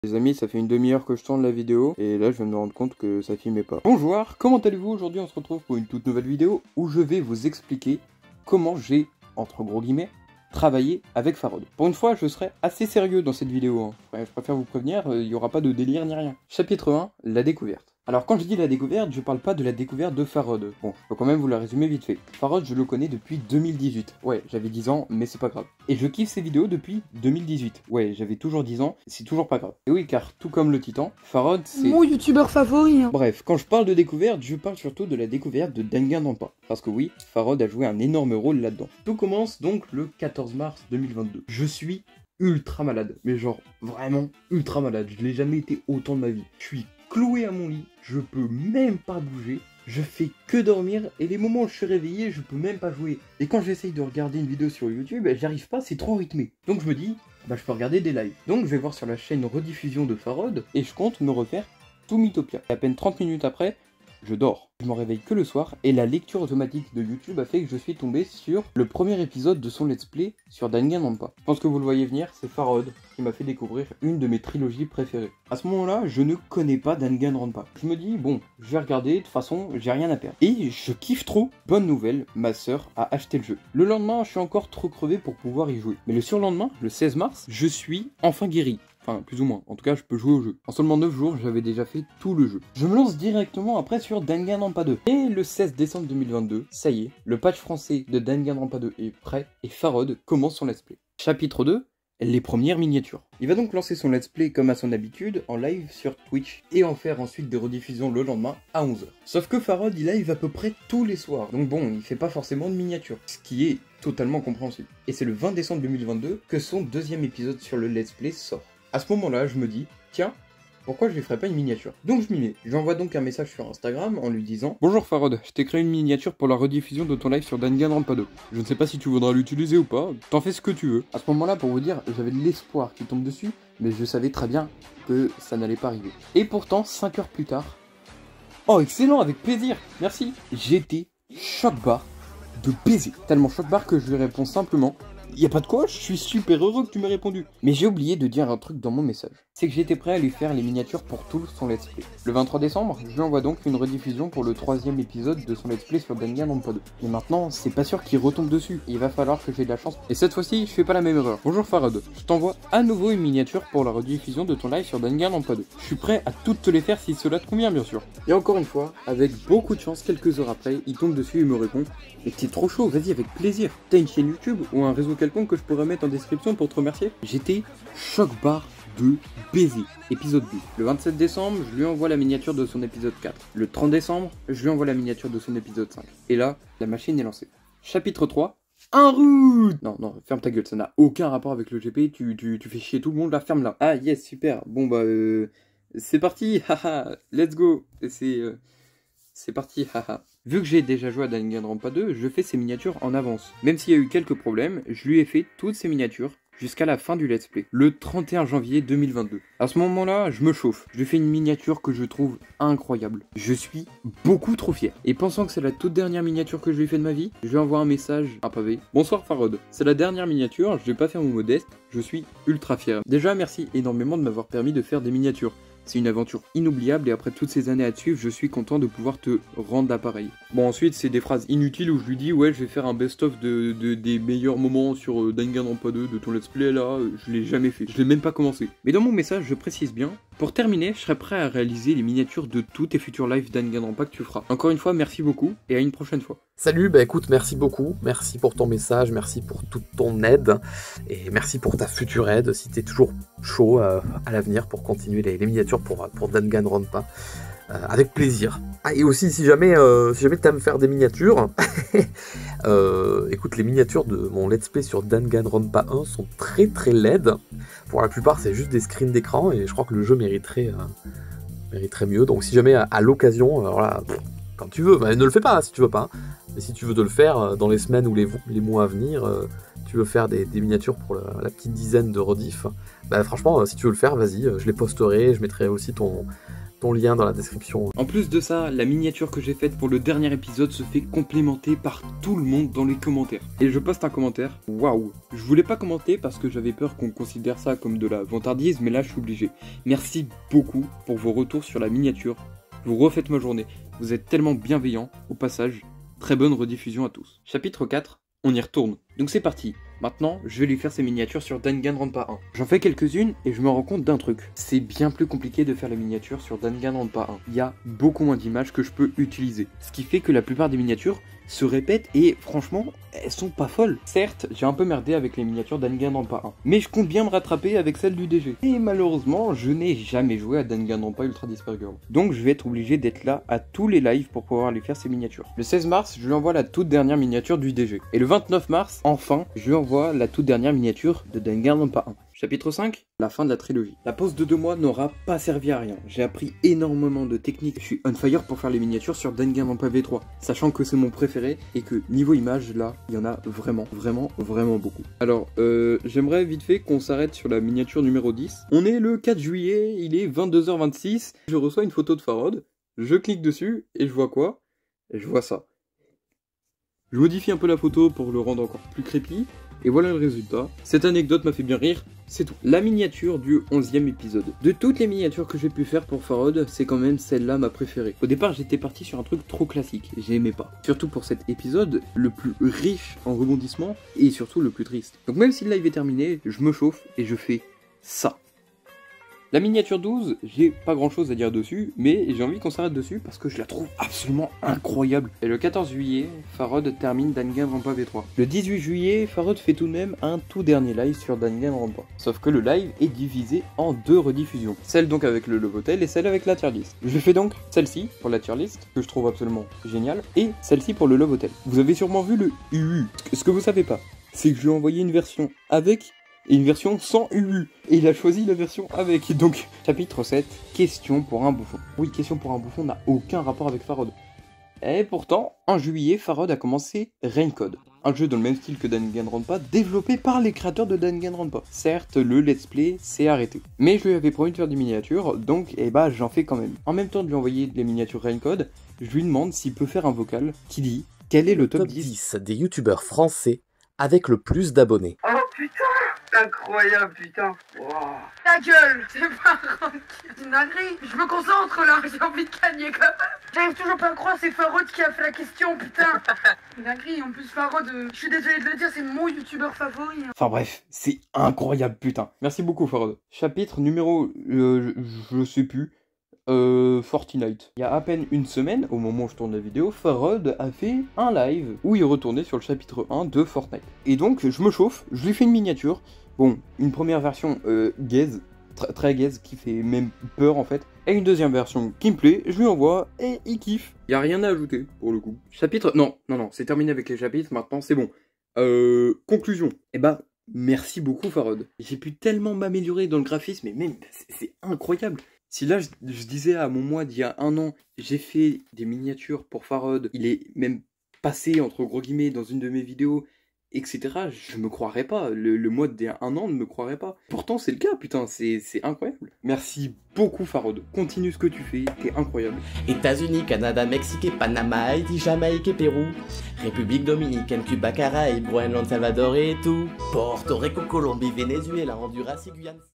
Comment j'ai travaillé avec Farod ? 💻 (C'est incroyable)
Salutation. (Pourquoi je parle comme un amiibo ?)
Aujourd'hui je vais vous raconter comment j'ai travaillé avec @FarodGames ! 🔥 (tah les pères castors)
Je précise que j'étais évidemment volontaire de lui faire des miniatures gratuitement, il n'a rien demandé à la base donc il n'a pas à payer ! ⚠️ (En plus j'ai déjà bien été récompensé, mais je vous spoil pas 🤫)
Bon visionnage !

Toutes les chaînes de Farod -
@Rafod (j'vais loin la)